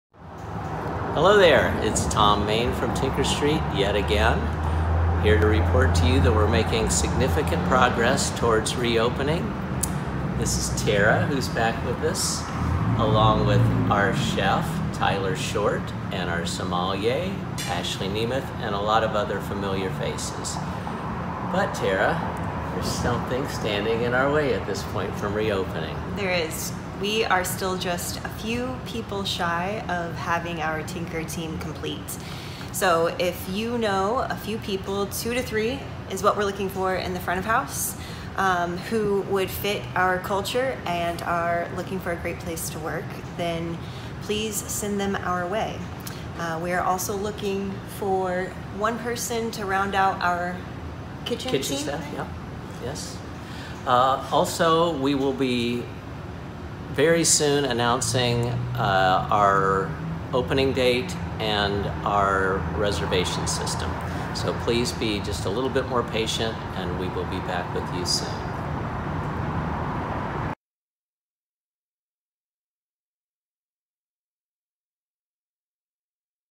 April 7, 2021 - Tinker Street Needs Your Help as it Nears Reopening
Tom Main and Tara Cummings are here to say -- your help is needed as we round out the staff.